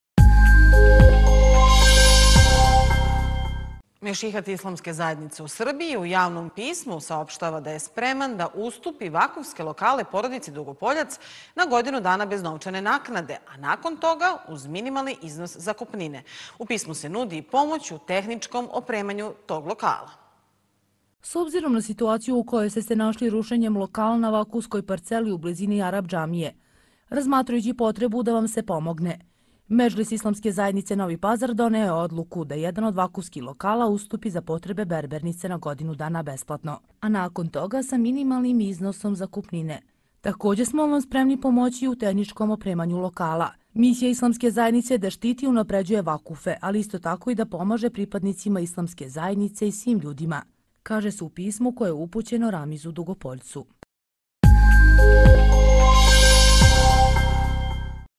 Mešihat nudi svoje lokale porodici Dugopoljac
"Mešihat islamske zajednice u Srbiji u javnom pismu usaopštava da je spreman da ustupi vakufske lokale porodici Dugopoljac na godinu dana bez novčane nadoknade, a nakon toga uz minimaln iznos zakupninine. U pismu se nudi i pomoć u tehničkom opremanju tog lokala